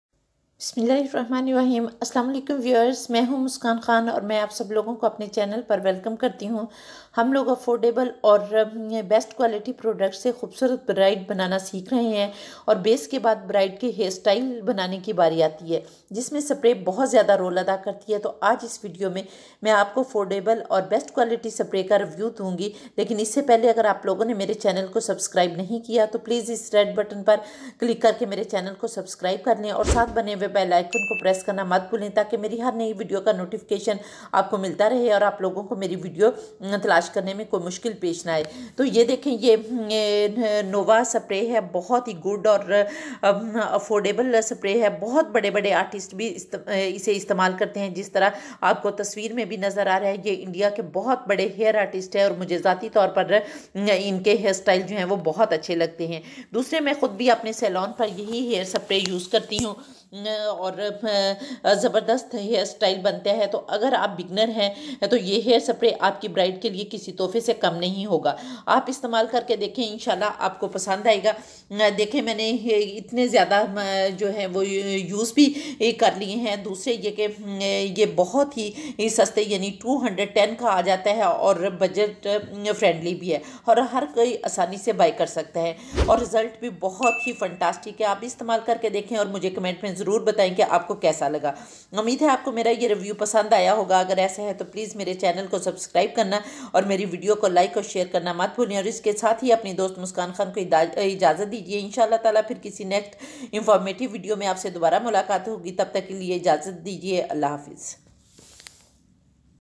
Review!Affordable And Best Quality Hair spray only 210||Nova Gold Super Firm Hold Hair Spray
Hi I am Muskan Khan and welcome to you our youtube channel MuskansTutorials
MuskansTutorials Review NovaGoldHairspray# SuperFirmHold
Nova Gold System Professionnel Super Firm Hold Hairspray - 400Ml
Fine Mist Evenly Coats Hair And Holds Style Firmly
No Coarseness, Maintains Style Even In Humid Weather
Gives Natural Shine To Hair
Images-source google .com